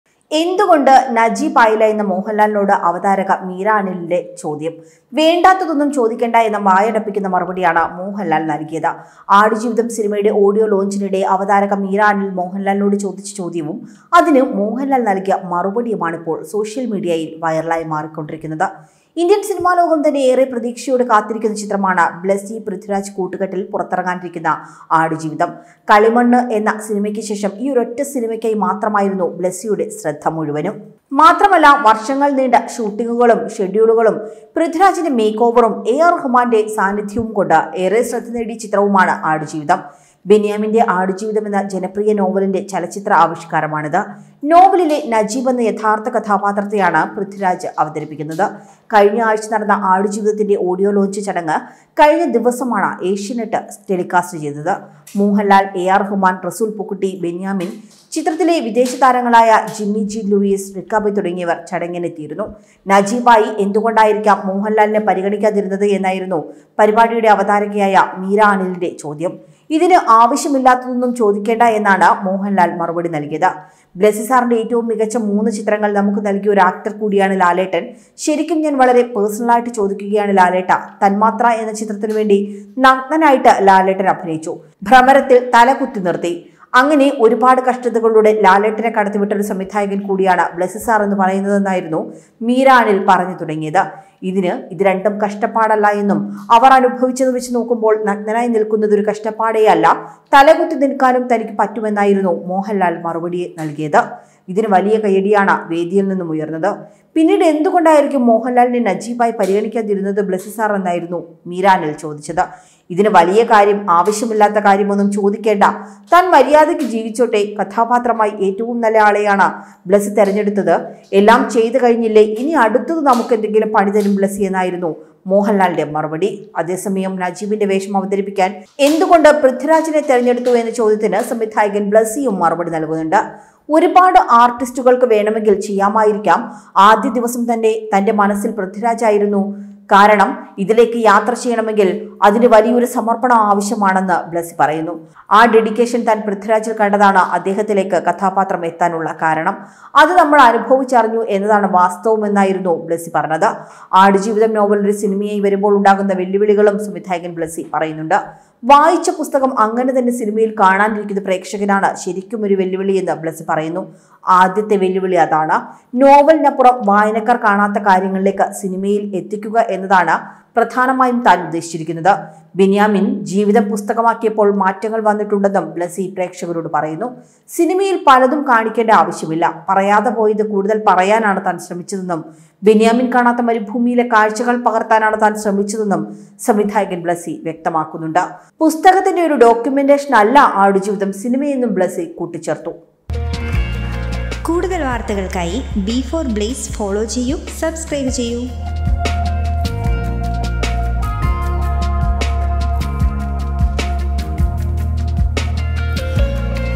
എന്ത് കൊണ്ട് നജീബായില്ലെന്ന് മോഹൻലാലിനോട് അവതാരക | Aadujeevitham
നജീബ് ആയി എന്തുകൊണ്ടായിരിക്കും മോഹന്‍ലാലിനെ പരിഗണിക്കാതിരുന്നത് എന്നായിരുന്നു പരിപാടിയുടെ അവതാരകയായ  മീരയുടെ ചോദ്യം. ഇതിന് ആവശ്യമില്ലാത്തതൊന്നും ചോദിക്കേണ്ട എന്നായിരുന്നു മോഹന്‍ലാലിന്റെ മറുപടി. '